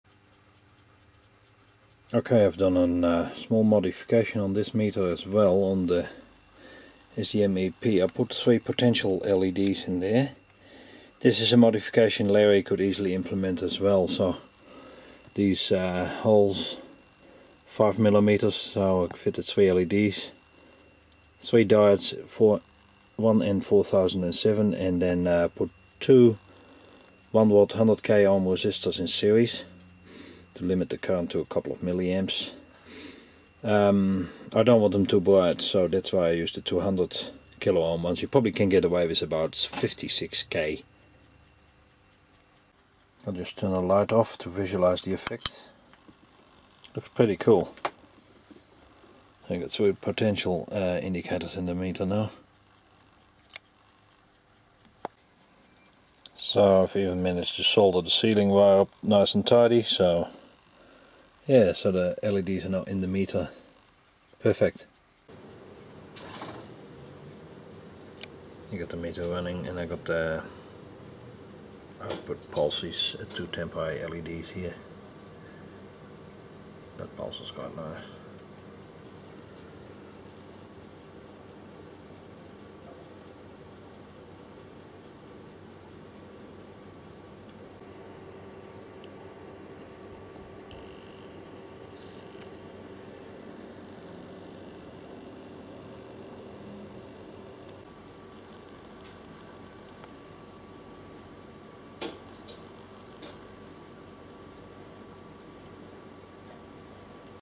LED Potential Indicators
Installed 3 LED's as potential indicators to populate the three 5 millimeter holes in the nameplate.
Meter SDMP E
Resistors 2 x 100 k.Ohms in series and 1N4007 diode. LED per phase.